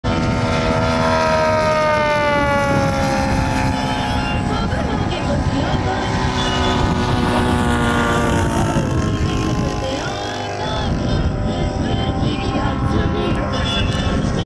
Corsair de Vincent.
Courte video d'un passage plein badin du fameux Corsair, lors d'un vol lors des Flyings Legends 2019, montez le son...